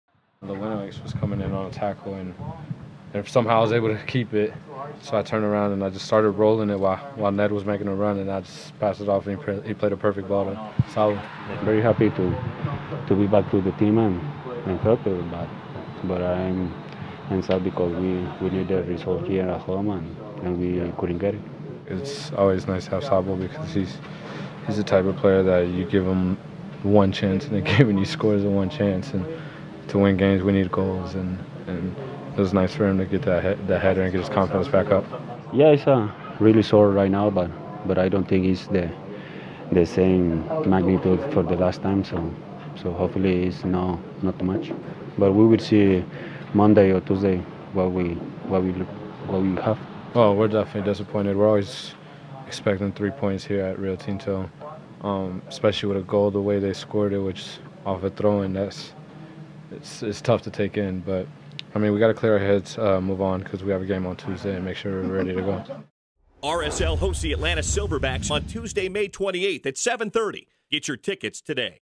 Real Salt Lake vs Chicago Fire, Postgame Reactions: Alvaro Saborio & Sebastian Velasquez
Real Salt Lake forward Alvaro Saborio and midfielder Sebastian Velasquez comment on the team's 1-1 draw with the Chicago Fire and Saborio's goal which opened the scoring for RSL in the 78th minute.